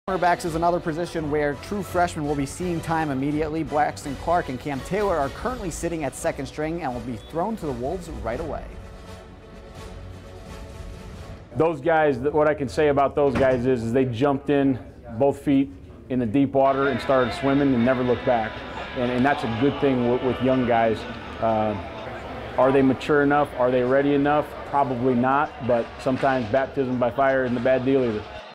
Freshman corners to see field right away
Freshman cornerbacks will see the field right away for Nebraska